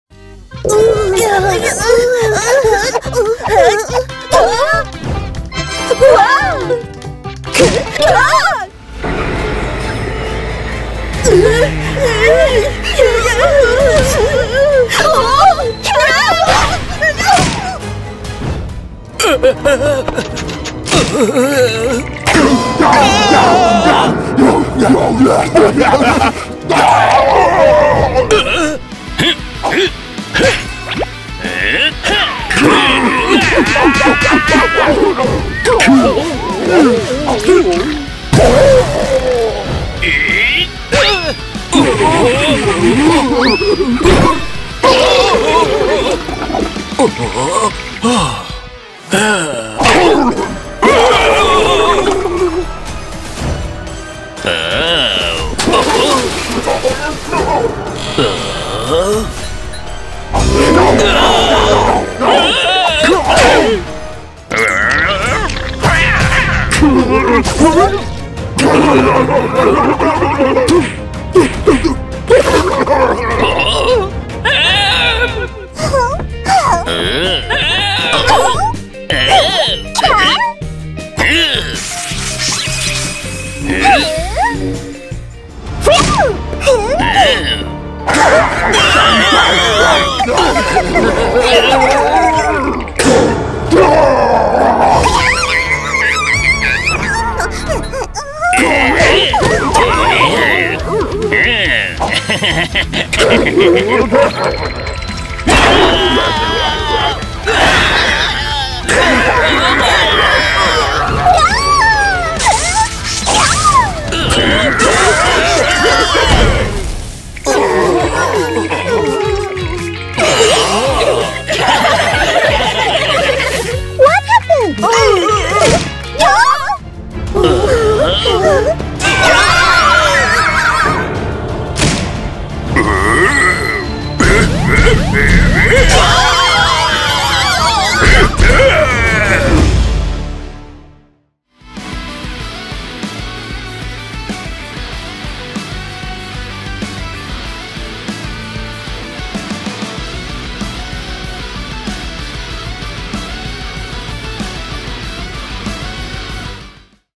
[ANIMATION] Hero BanBaleena, With Tamataki & Chamataki | Garten Of Banban 3 Cartoon
[ANIMATION] Hero BanBaleena, With Tamataki & Chamataki?! | Garten Of Banban 3  Cartoon   | Slime Cat

Today I show you an animation that talk about "Hero BanBaleena, With Tamataki & Chamataki TOILET?!".

[Characters]

Hero BanBaleena
Tamataki & Chamataki
Jumbo Josh
Banban
Nabnab

Wubbox

[Garten Of Banban ANIMATION - "Hero BanBaleena, With Tamataki & Chamataki TOILET?!" ]